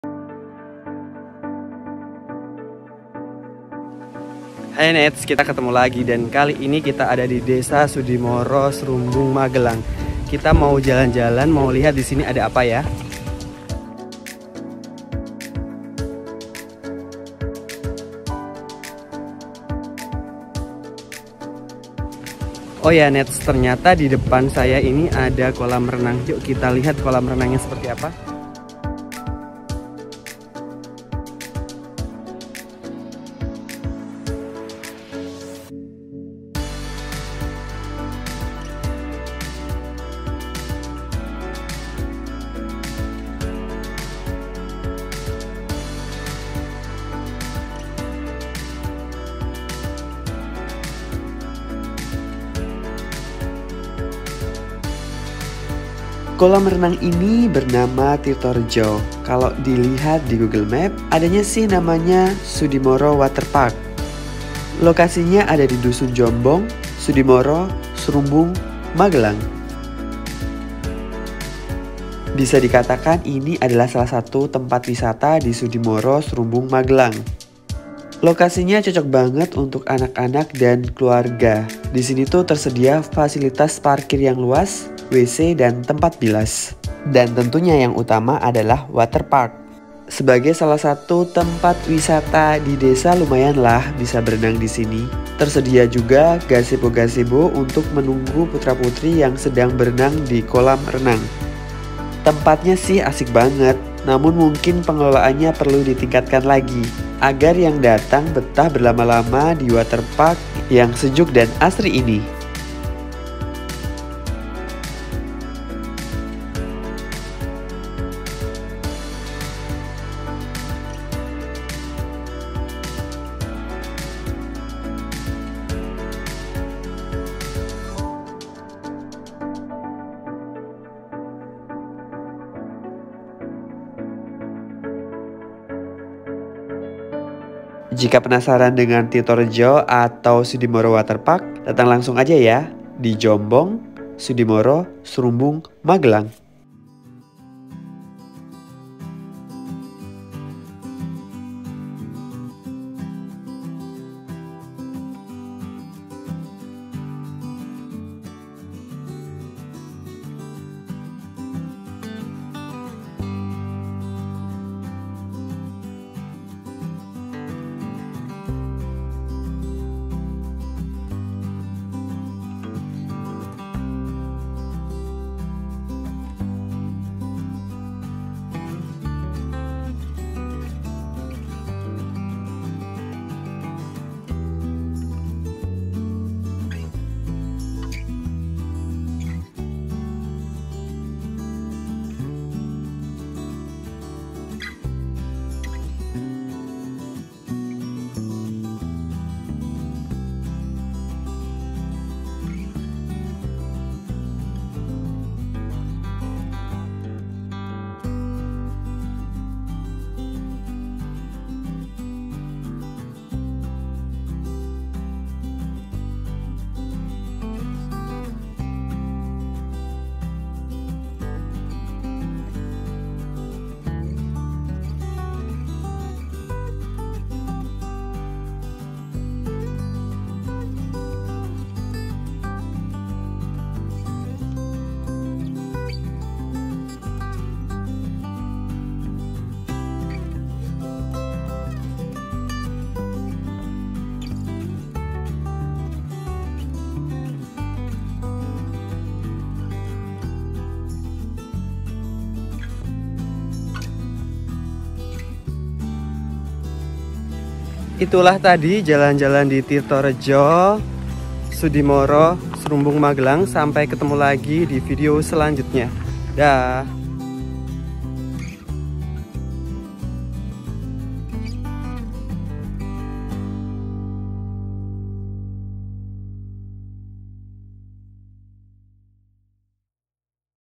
Tirto Redjo, Waterpark di Sudimoro, Srumbung, Magelang
Hi Netz

Udah nentuin belum liburan kemana? atau sama seperti saya di rumah aja?

Beberapa waktu lalu saat berada di Sudimoro, Srumbung, Magelang tanpa sengaja menemukan waterpark ini. Namanya Tirtoredjo atau Waterparknya Sudimoro

kalau penasaran langsung check TKP aja ya Netz